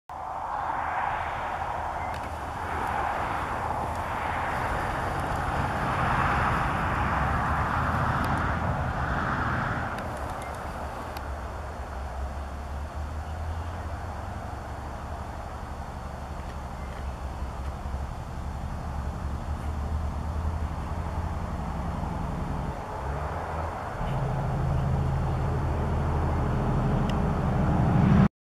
Canon SX50 HS Zoom Test
Test of super zoom on the Canon SX50 HS